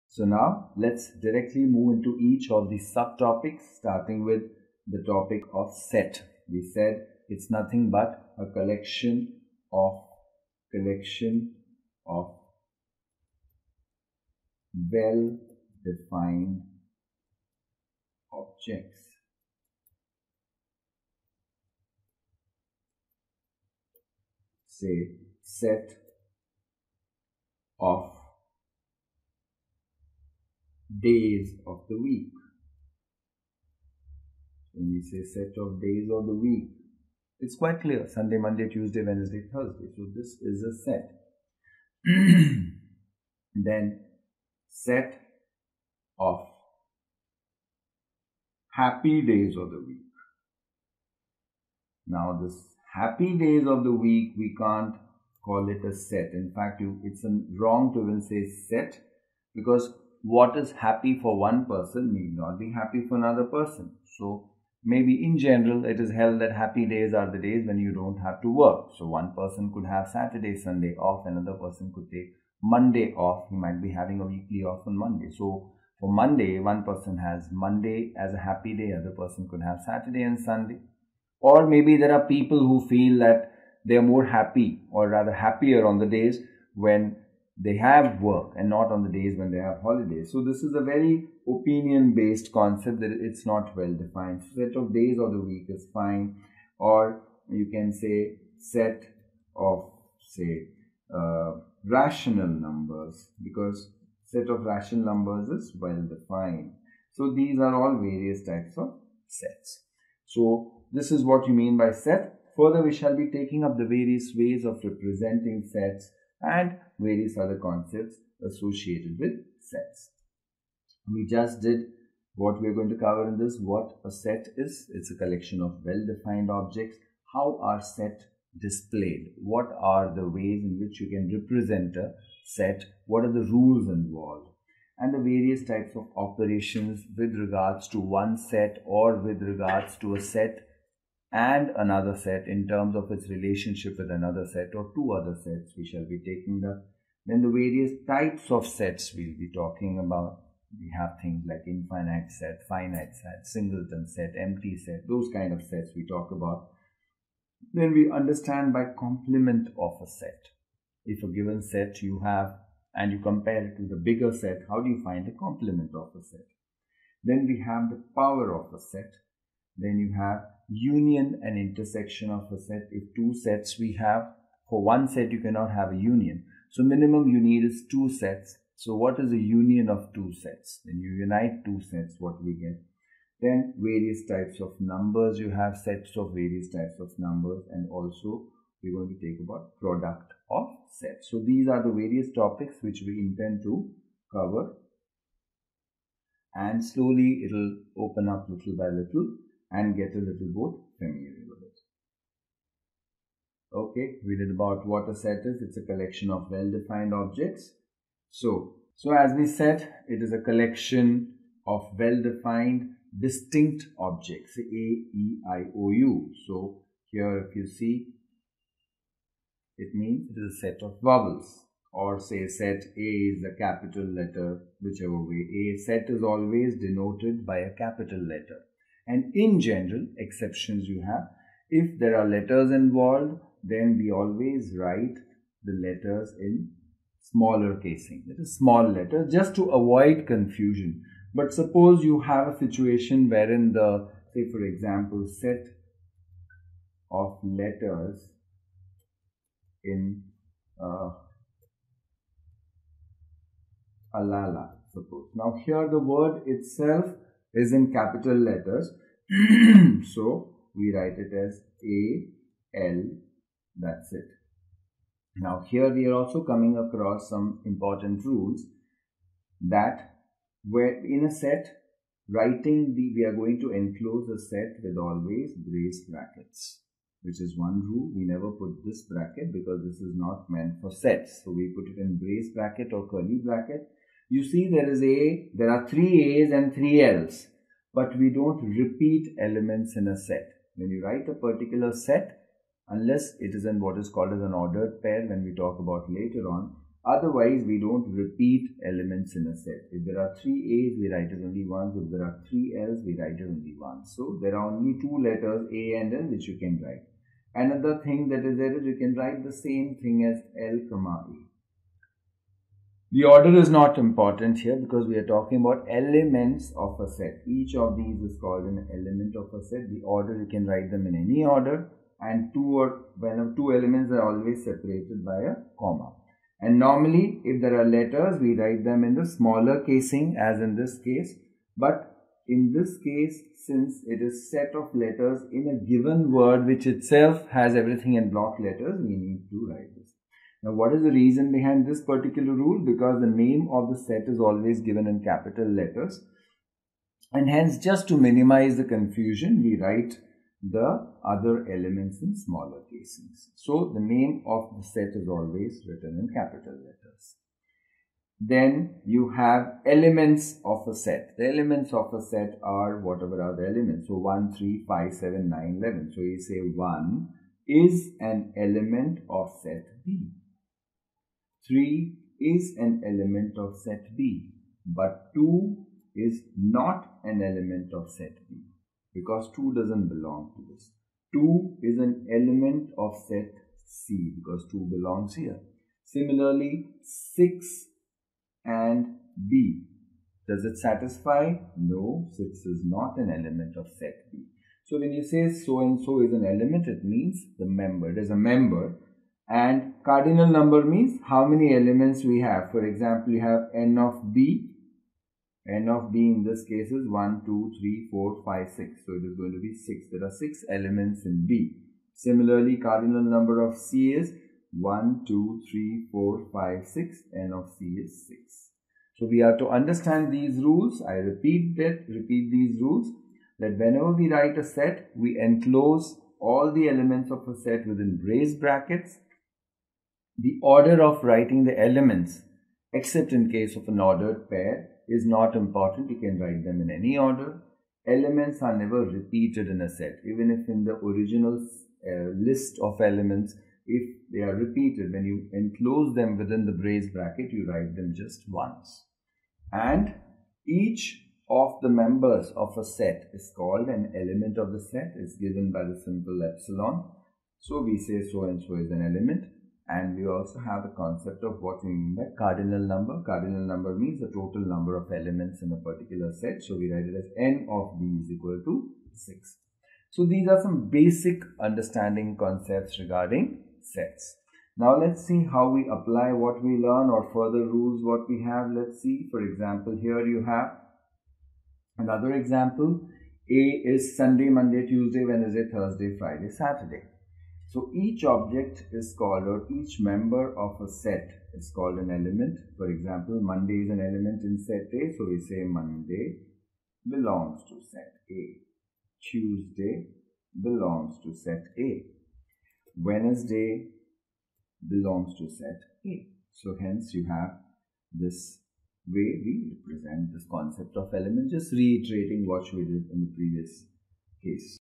What is Set | Set, Function, Relationship & Venn Diagram | CA CPT | CS & CMA | Class 11
What is Set, Learn Set, Function & Relationship. What is Set, How are sets displayed, Properties of Union & Intersection, What is relationship and Function.